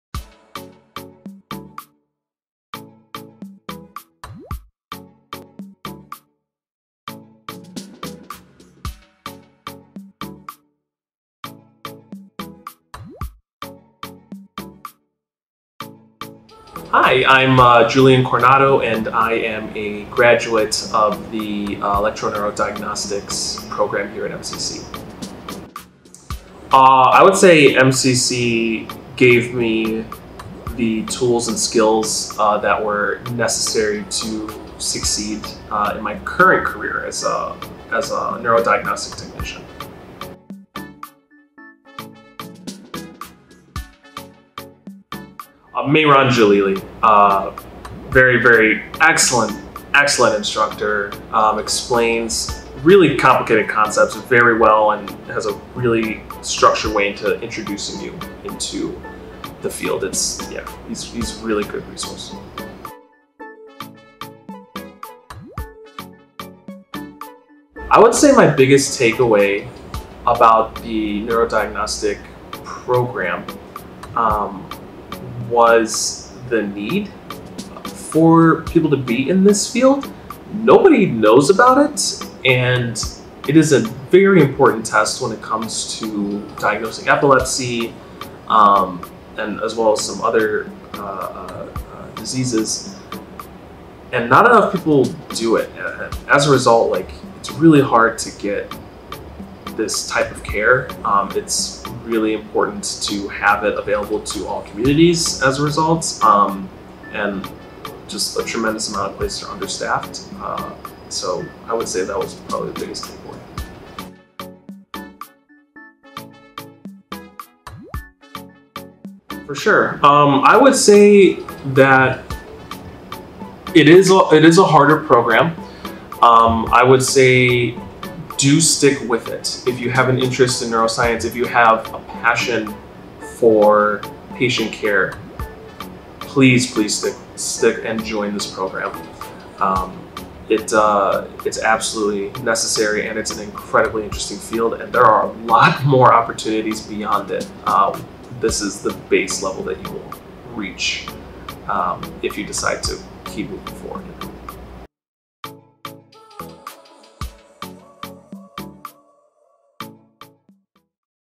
SUCCESS STORY: Electroneurodiagnostic (END) Technology Graduate - Julian C.
Julian Coronado is a graduating student of Midwestern Career College in Electroneurodiagnostic (END) Technology. He recently completed his externship and is now working as a  Neurodiagnostic Technologist. We asked Julian how MCC helped his career, check out what he had to say.

Timecodes:
0:06- How has MCC helped you?
0:41 - Q: The person that helped you the most in your academic journey was…
1:03 - Q: What is the biggest takeaway that you learned from your time in the program?
1:54 - Q: What would you tell a future student either about the program or studying at MCC